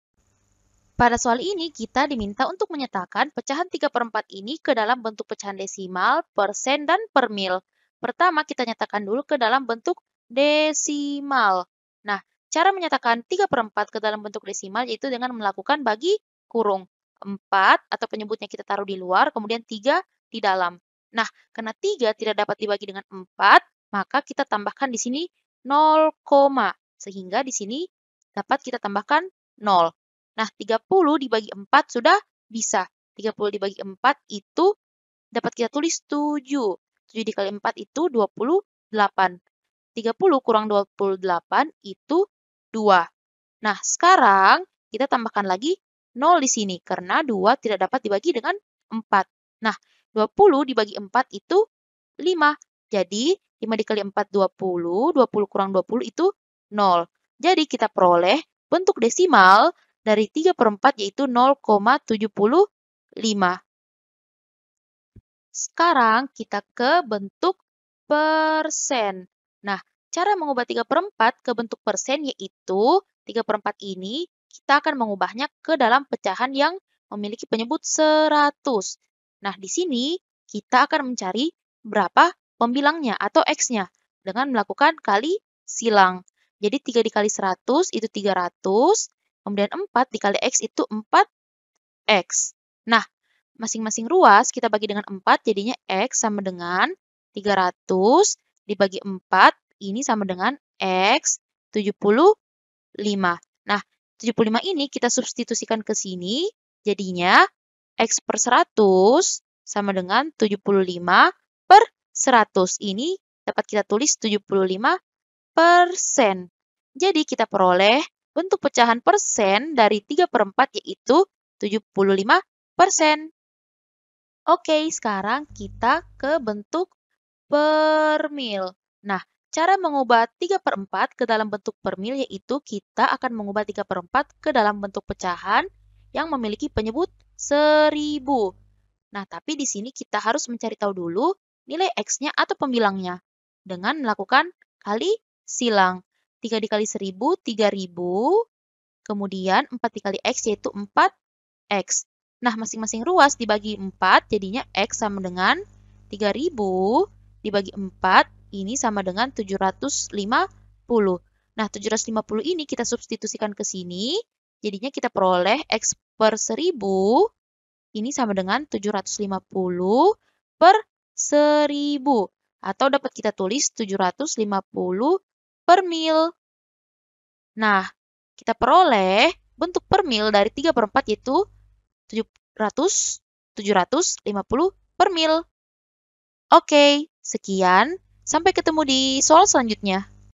Nyatakan pecahan-pecahan berikut dalam bentuk pecahan desimal, persen, dan permil. 3/4
Topik atau Materi: Pecahan Desimal dan Persen - BILANGAN BULAT DAN PECAHAN - BILANGAN - Matematika Kelas 7 SMP - K-13 Revisi

Sumber Soal: Matematika 1 kelas 7 K-13 revisi, MRJ 2018 , Erlangga

00:00 : Bedah Soal
01:00 : Konsep, rumus dan pengertian Pecahan Desimal dan Persen
01:18 : Langkah penyelesaian soal
02:10 : Jawaban akhir
02:40 : Penutup